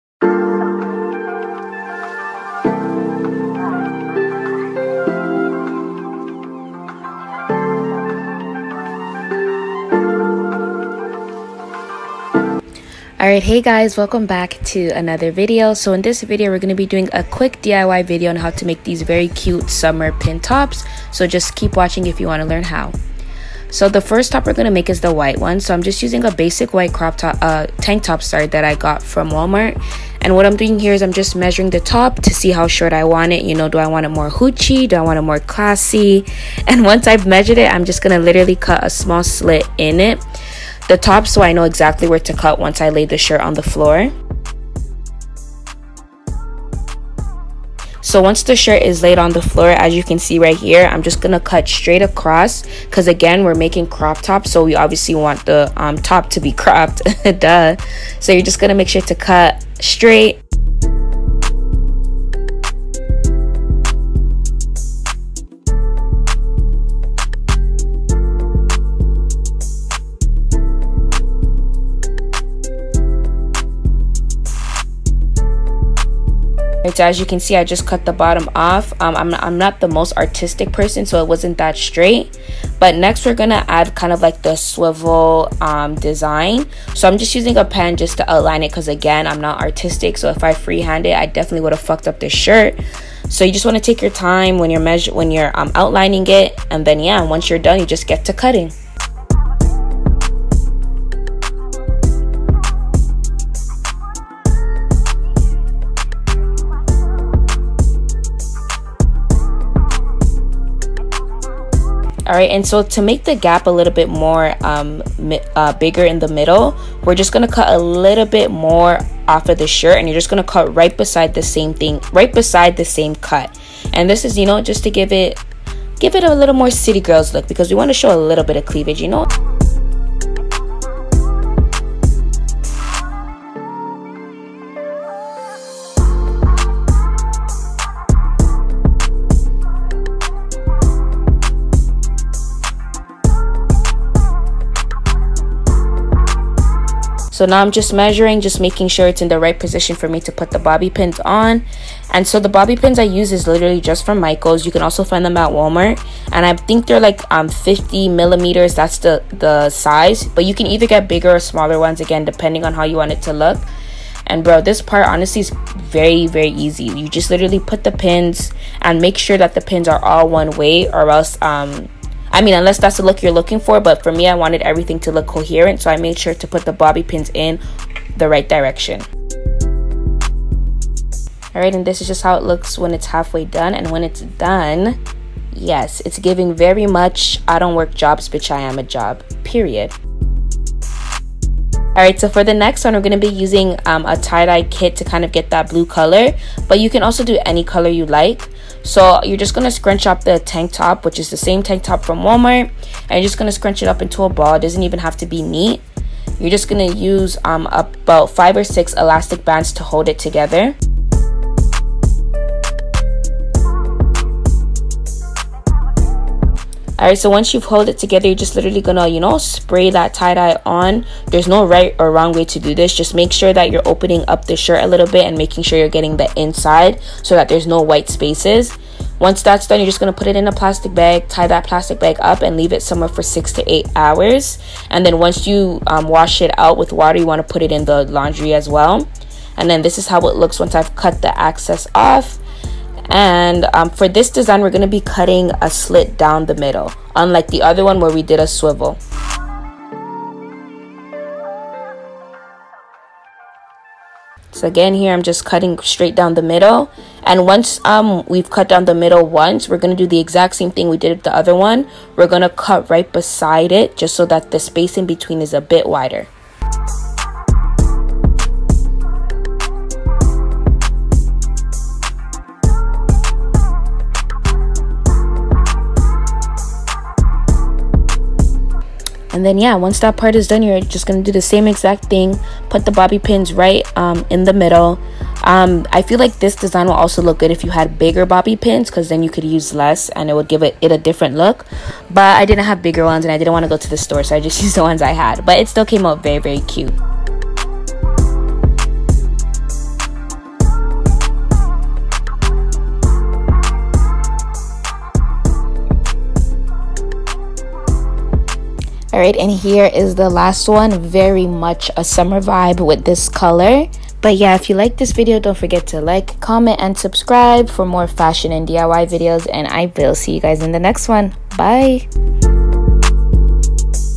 DIY SAFETY PIN CROP TOP | HOW TO MAKE SAFETY PIN TOP | TIE DYE CROP TOP
hope everyone is trying to have the best summer they can while staying safe ! in this video I show u guys how to make the cutest summer tops. while you are here you might as well subscribe for move videos !!